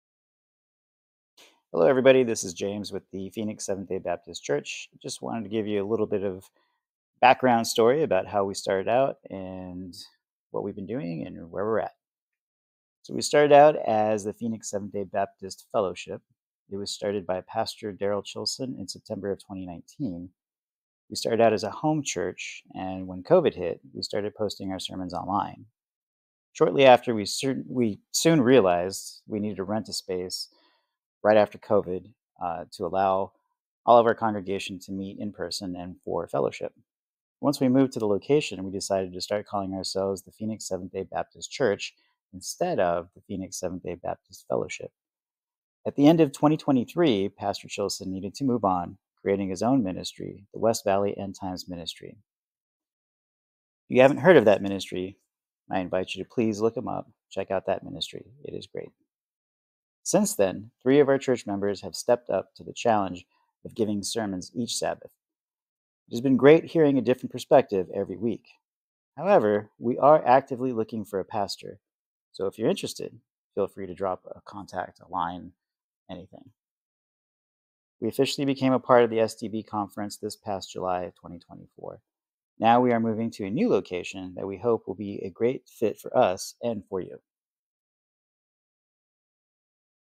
Phoenix Seventh Day Baptist History
This video tells a brief history of our Phoenix church, telling you how we started all the way through where we are today.